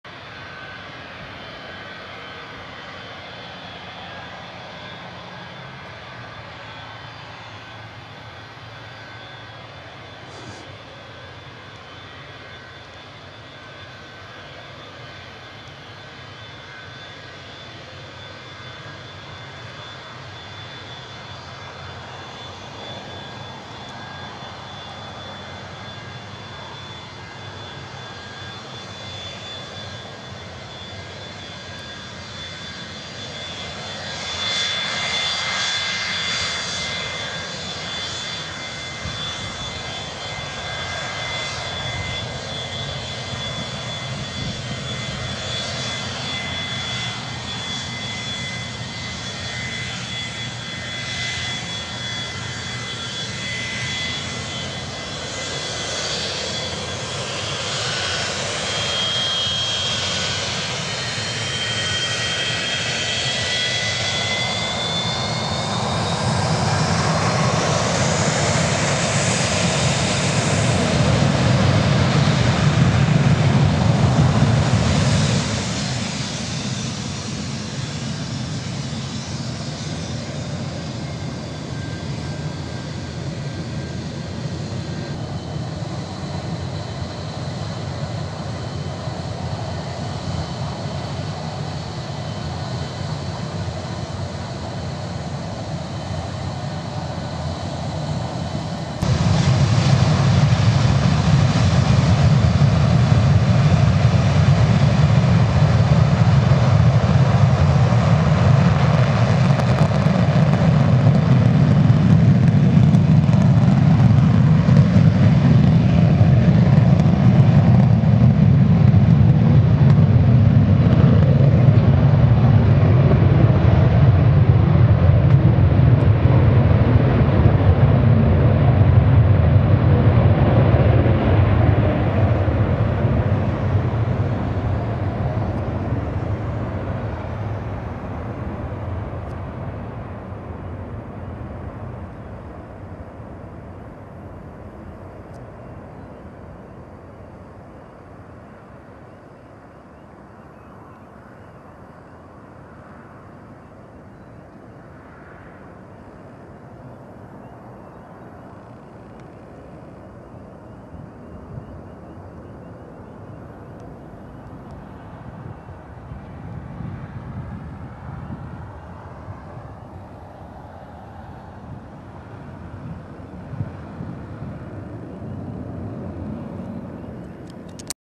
Air Inuit | Boeing 737-200 rejected take-off | C-GMAI | Val-d'Or (CYVO)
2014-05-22 "Inuit 353" rejected take-off and successful 2nd attempt